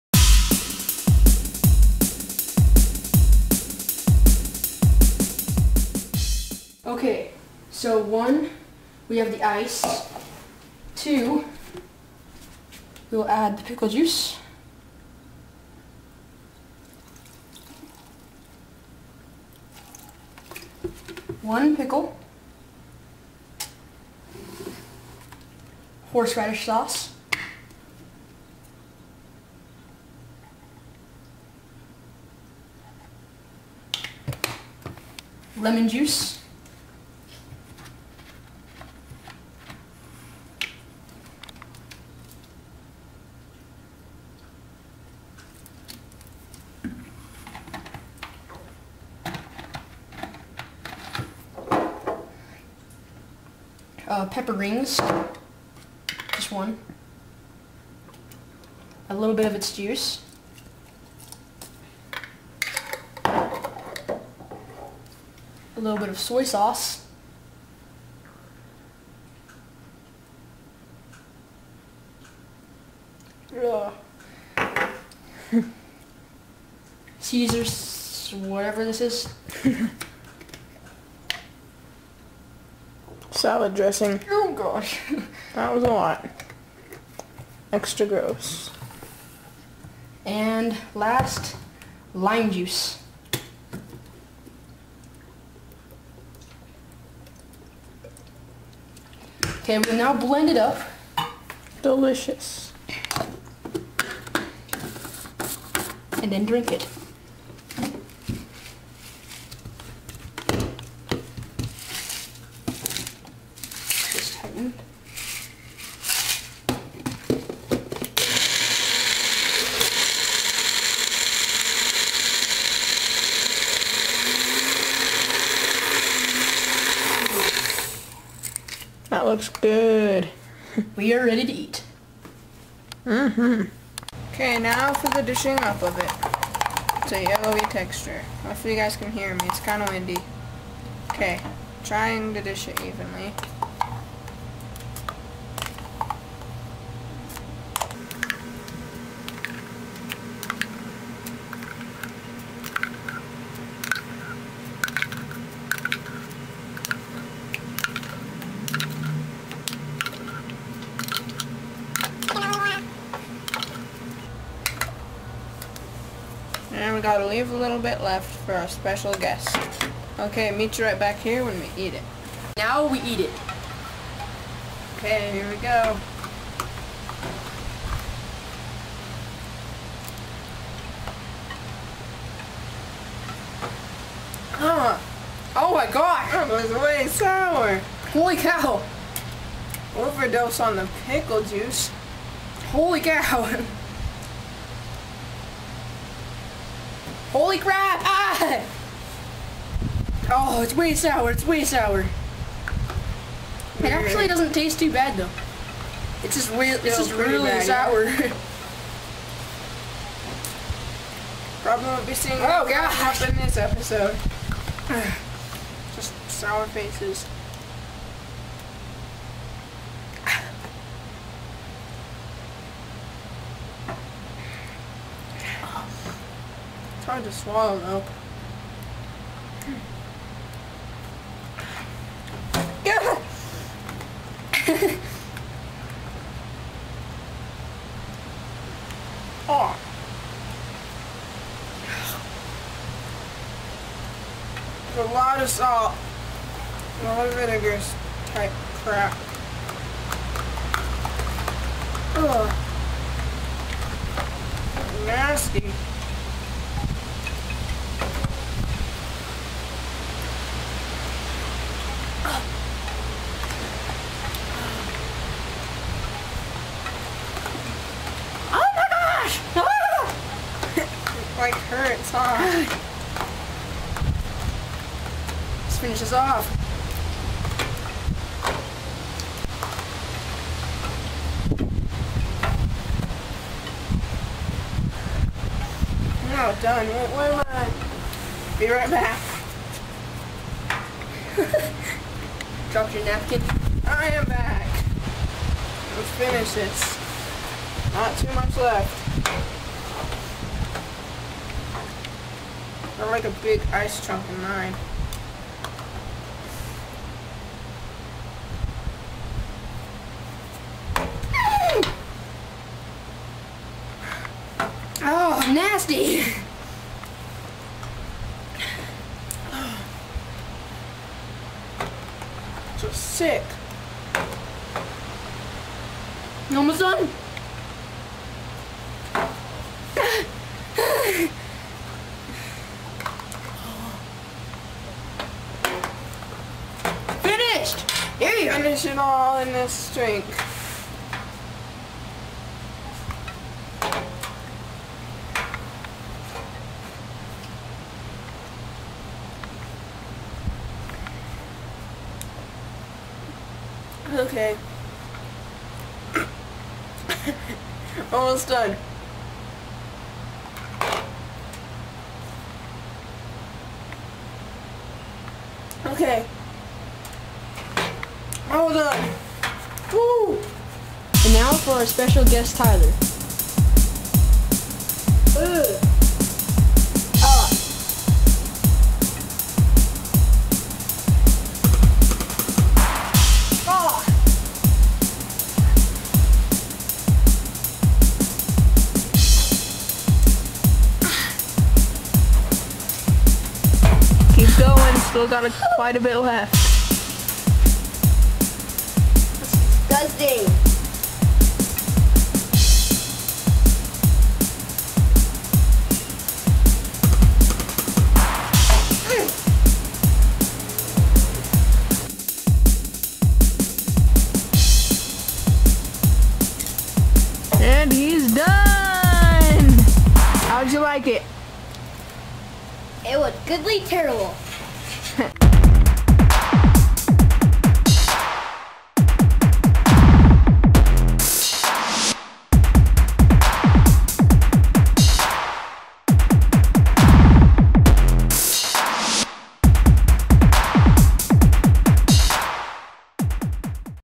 EWF - Sour Slushie
ever thought of a slushie with pickles in it? we did